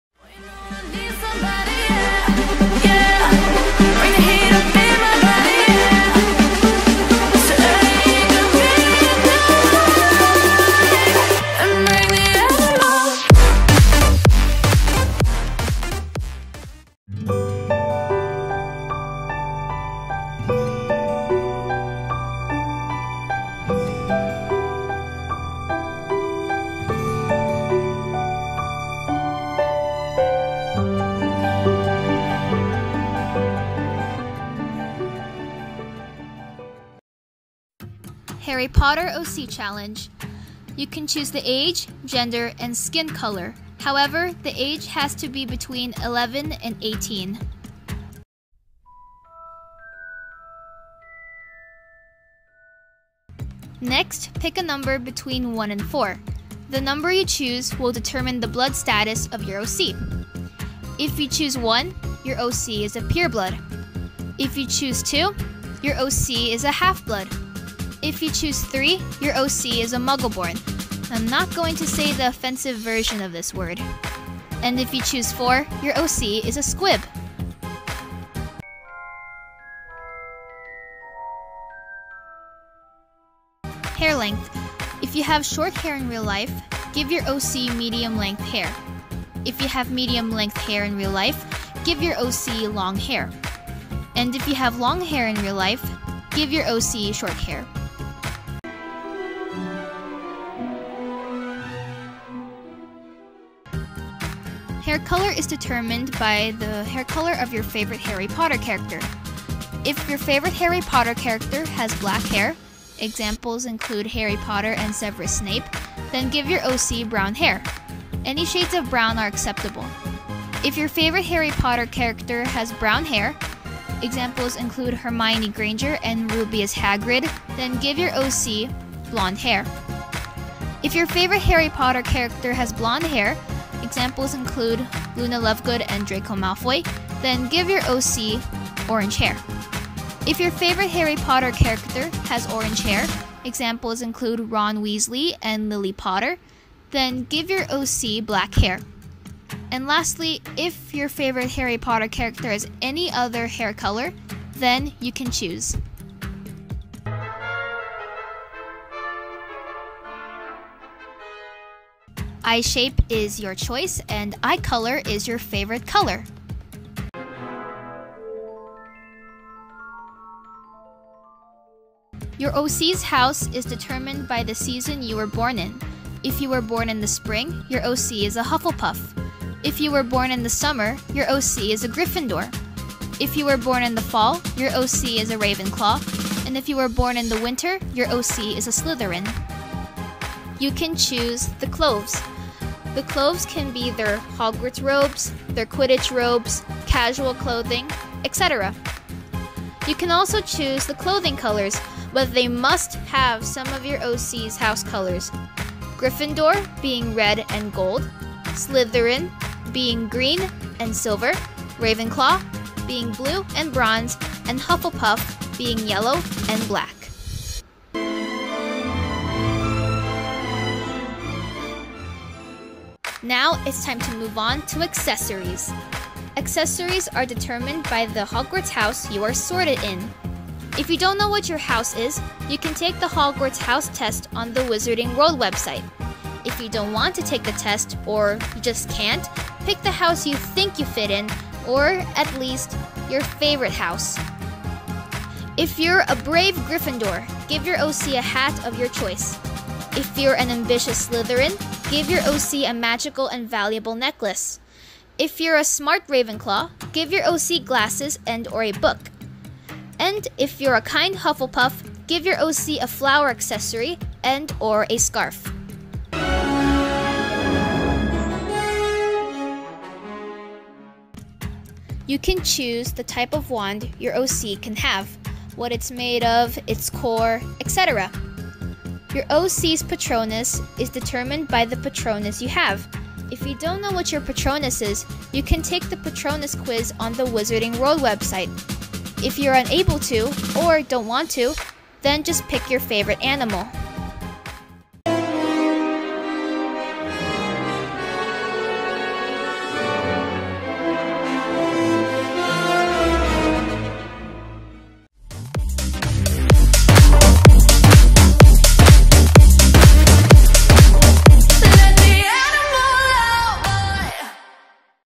Harry Potter Oc Challenge|Gacha Club|Butterfly Warrior Edits
I hope you guys like my oc. I don’t really have any extra info on her yet. The video on the top I’m following belongs to @TheMusicArtist24. Go check her channel out below.